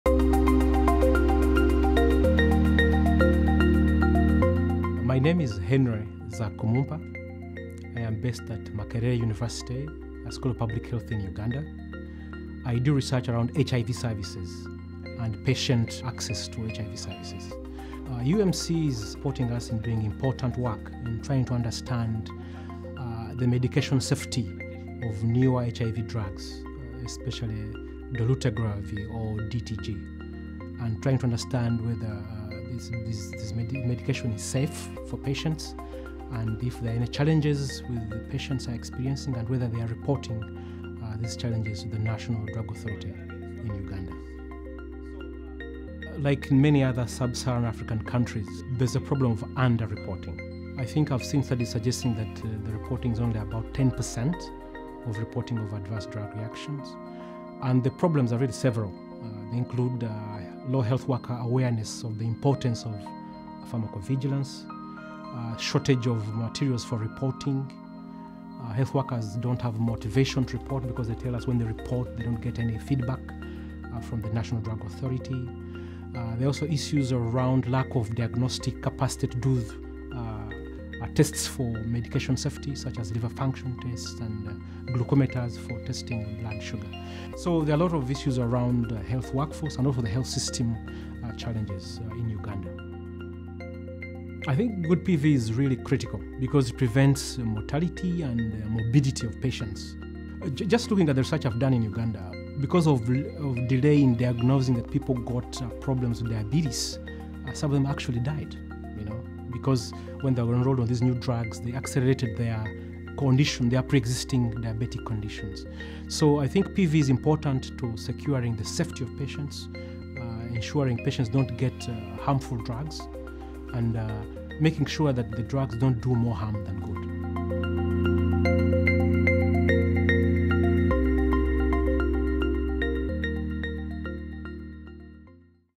Henry Zakumumpa on PV in Uganda
For five years now, UMC has worked with the Consortium for Advanced Research Training in Africa (CARTA) to fund pharmacovigilance research in Africa.

We are the proud sponsor of researchers like Dr Henry Zakumumpa of Makerere University School of Public Health in Kampala, Uganda whose evaluation of new HIV treatments and care underlines the need for robust safety monitoring.